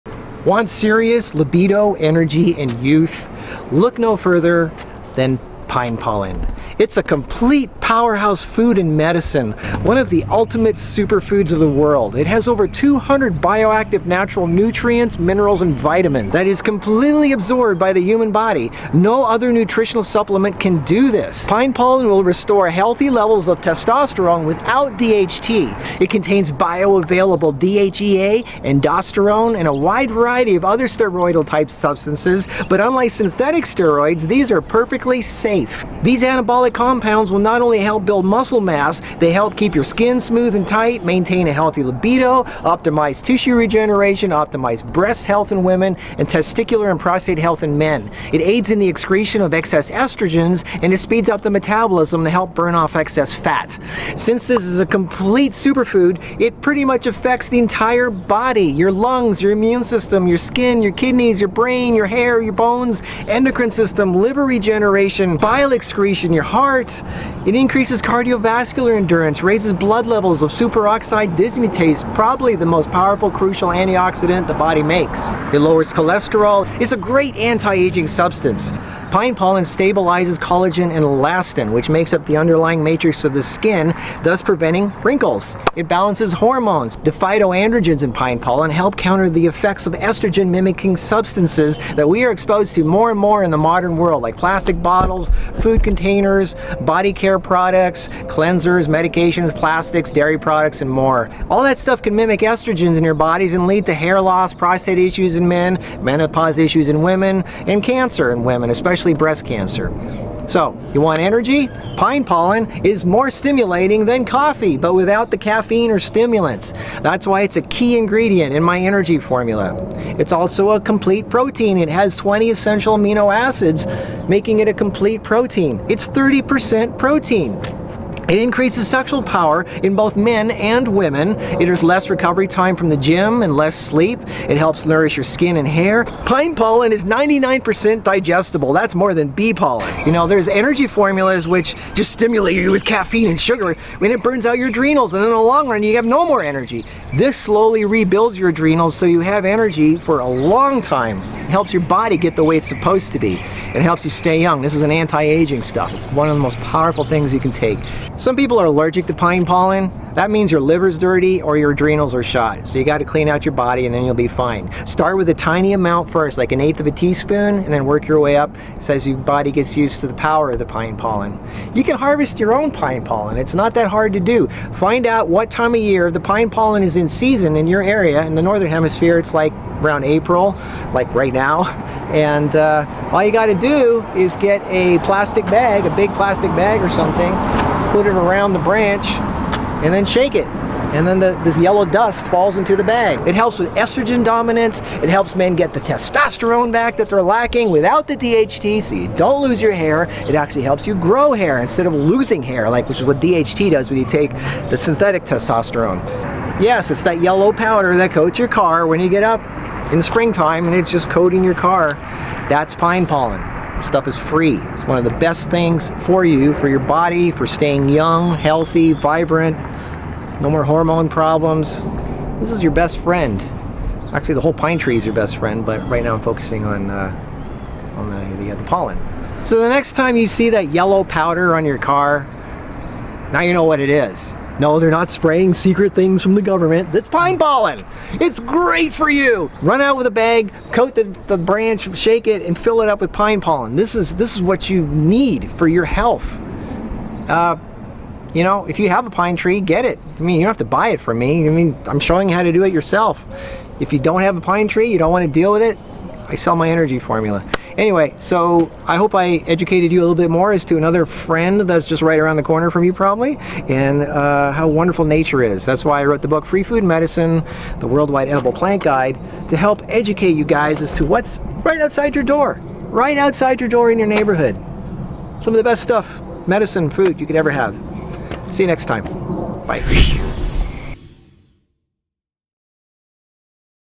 Pine Pollen: Nature's Safe Hormone Source HRT, DHEA, Natural T
Forget unsafe steroids and hormone replacement therapy and testosterone shots- nature has something much better- pine pollen.
A complete protein and energy source, pine pollen is a true superfood. Pine pollen is a great all natural hormone source for those not wanting to take steroids. Great for aldosterone, DHEA, libido, sex drive, SOD (superoxide dismutase) so obviously its good for natural bodybuilders, vegan, muscle, energy for the gym, working out, a natural source of testosterone, helps with collagen, elastin, great for breast health and getting rid of xenoetstrogens and estrogen. You could say this was nature's natural steroid.

Pine Pollen is very nutritionally dense. It contains over 200 natural nutrients, minerals and vitamins. It can be used for long periods of time without any toxic effects. One of the attributes of this tonic medicine is that it is adaptogenic meaning that it will cater to exactly what your body needs and treat areas that are in distress. Pine pollen is a whole food and has many medicinal properties and effects most of the entire body. It will increase your energy level and your mental integrity. Whatever nutritional regimen you follow, you will quickly discover that Pine Pollen is a super food and one that will greatly benefit you.
Pine Pollen “a great anti-aging substance”
Abstract
The present paper was designed to investigate the effect of pine pollen against aging in human diploid fibroblast 2BS cells and in an accelerated aging model, which was established by subcutaneous injections with D-galactose daily for 8 weeks in C57BL/6J mice. Pine pollen, 1 mg/mL and 2 mg/mL, is proved to delay the replicative senescence of 2BS cells as evidenced by enhanced cell proliferation, decreased SA-β-Gal activity, and reversed expression of senescence-associated molecular markers, such as p53, p21Waf1, p16INK4a, PTEN, and p27Kip1 in late PD cells. Besides, pine pollen reversed D-galactose-induced aging effects in neural activity and inflammatory cytokine levels, as indicated by improved memory latency time and reduced error rate in step-down test and decreased concentrations of IL-6 and TNF-α in model mice. Pine pollen inhibited D-galactose-induced increment of AGEs levels thus reversed the aging phenotypes in model mice. Furthermore, the declined antioxidant activity was obviously reversed upon pine pollen treatment, which may account for its inhibitory effect on nonenzymatic glycation, NEG, in vivo. Our finding presents pine pollen as an attractive agent with potential to retard aging and attenuate age-related diseases in humans.

Abstract
Unconjugated vitamin D and its metabolites were investigated in the pollen of Pinus nigra Ar. and Pinus sylvestris L. by TLC, HPLC and competitive radiochemical determination of 25-hydroxycholecalciferol, 25-OHD3.  It was found that vitamin D,  D2, D3, was present in the pollen in amounts about 2 micrograms/10 g and 25-OHD3, 24,25-dihydroxycholecalciferol, 24,25- OH, 2D3, and 1,25-dihydroxycholecalciferol, 1,25-OH, 2D3, between 0.1 and 3 micrograms/10 g of pollen, dependent on pollen species and methods.

The recent research on chemical components and pharmacological function of pine pollen is summarized.Results show that pine pollen contains protein,aminophenol,mineral,vitamin,enzyme,coenzyme,fattiness,flavone,nucleic acid,monosaccharides,amylose and phospholipid,etc.Furthermore,pine pollen has many pharmacological functions,including anti tiredness,anti-senility,liver protection,improvement of cardio-cerebral blood vessel and sexual function,and so on.Meanwhile,the exploitation and utilization prospects are given.

It helps regenerate the body’s …tissues..bones..
These results suggest that the pine pollen might be beneficial in the treatment of chronic inflammatory disorders.